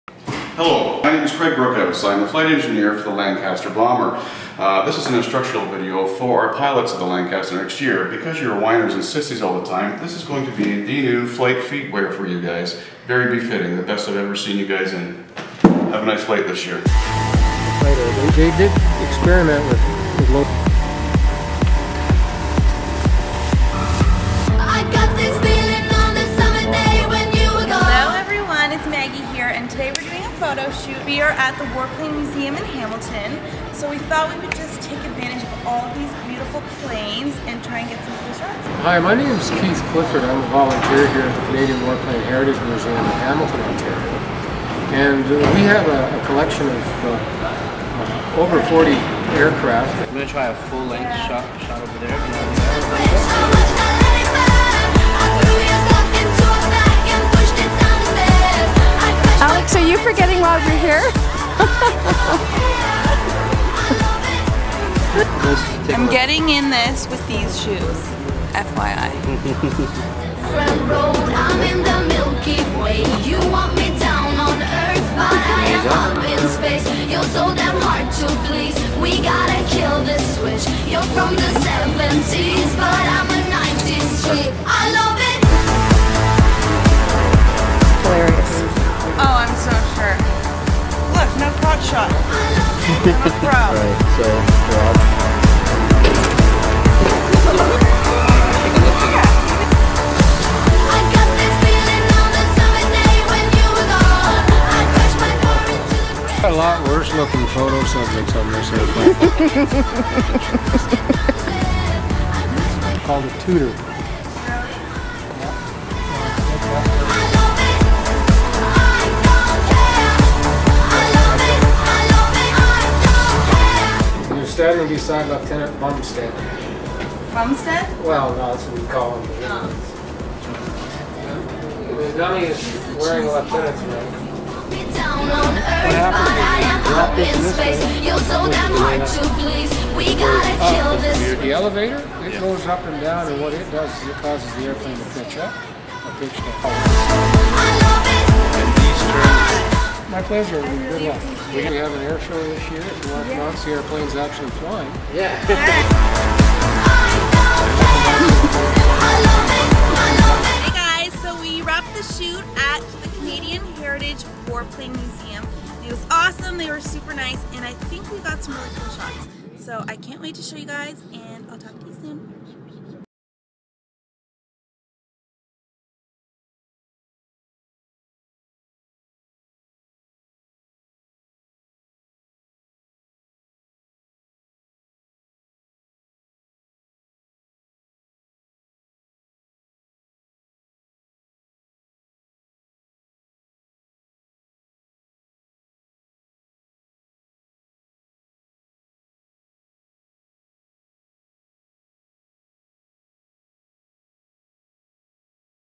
Airplane Photoshoot! (Vlogisode)
Hey everyone! Check out this photoshoot I did at the Canadian Warplane Heritage Museum in Hamilton, Ontario!

Thanks to the Canadian Warplane Heritage Museum for having us, and to Keith Clifford for being such a knowledgable guide.